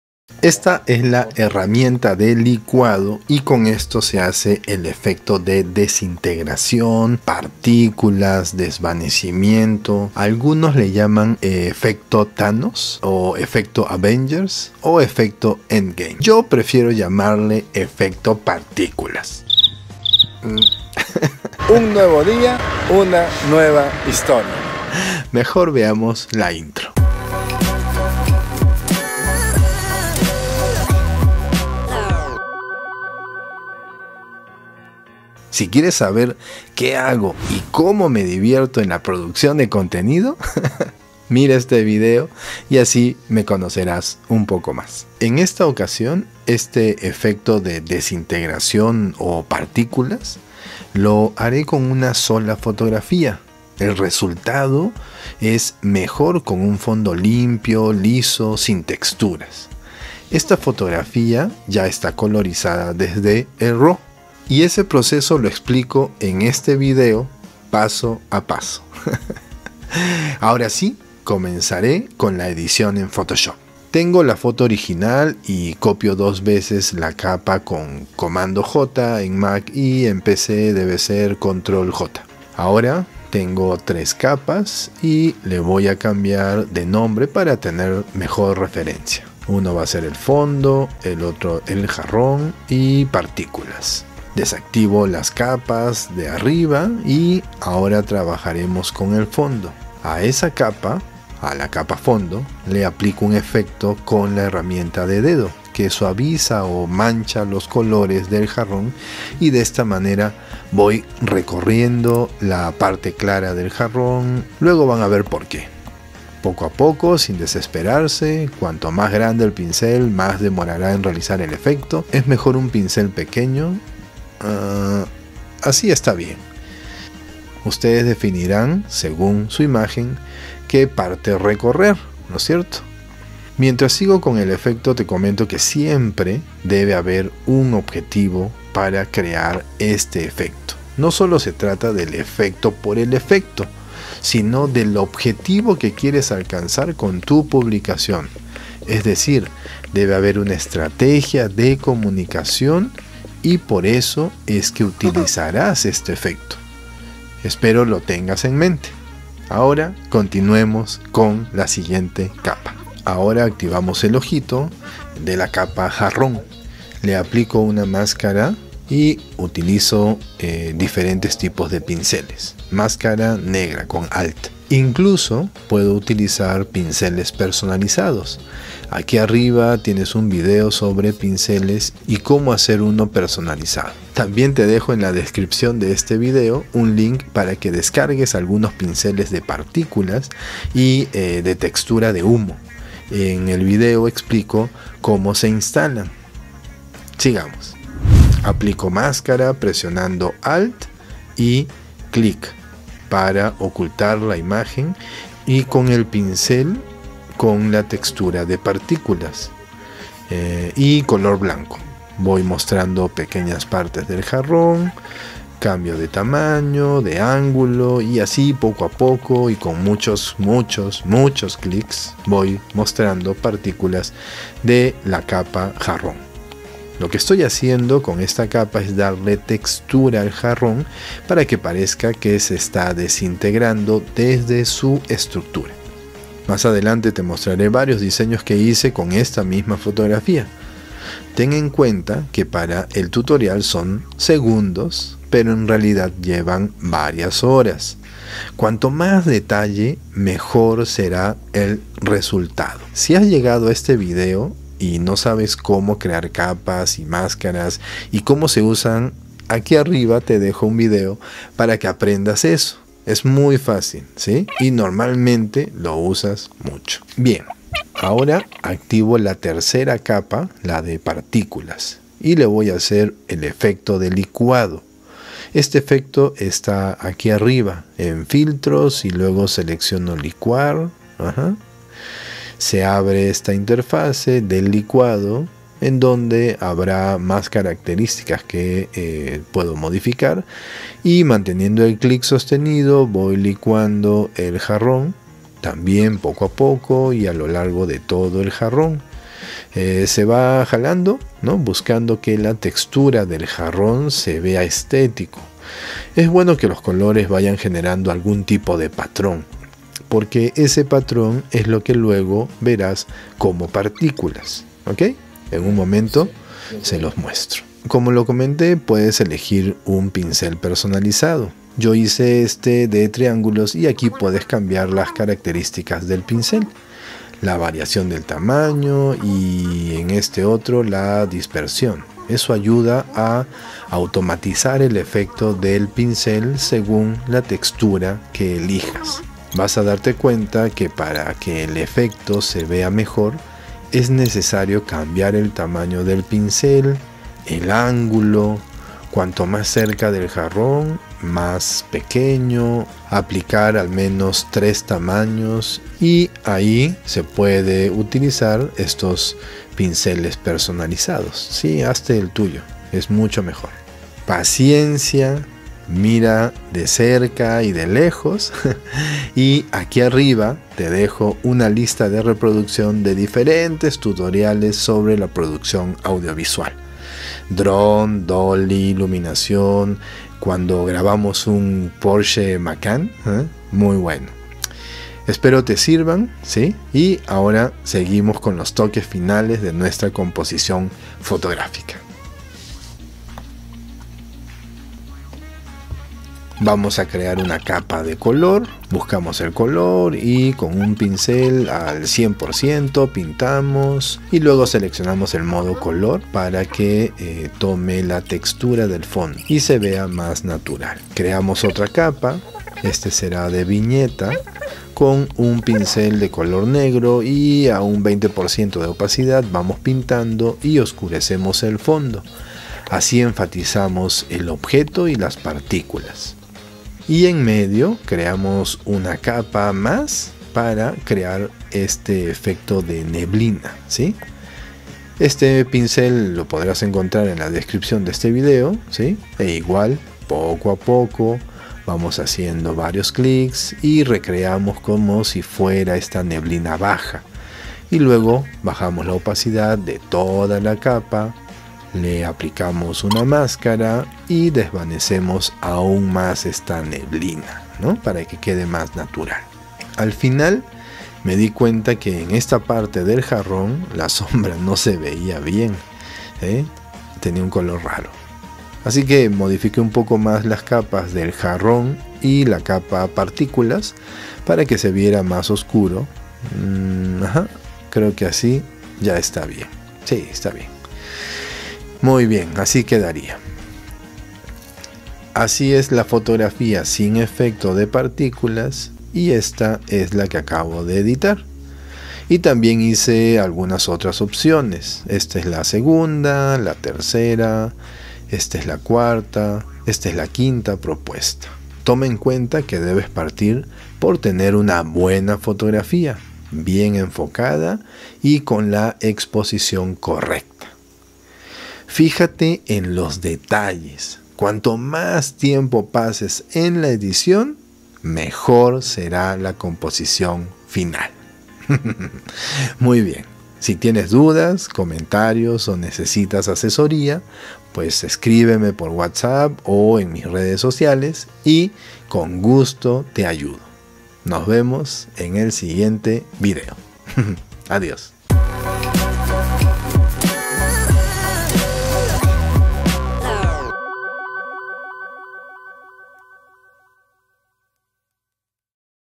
Cómo hacer efecto PARTICULAS (SÚPER FÁCIL) FX AVENGER THANOS
😁 Aprende el FX de Desintegración Partículas Licuado de una fotografía en PHOTOSHOP, paso a paso, en este tutorial completo, además te dejo pinceles personalizados y la imagen con la que hice este ejercicio.
Si descargas alguno de estos recursos, por favor deja algún comentario, gracias!
😱Muy pronto CURSO de FOTOGRAFÍA GRATIS, prontito :)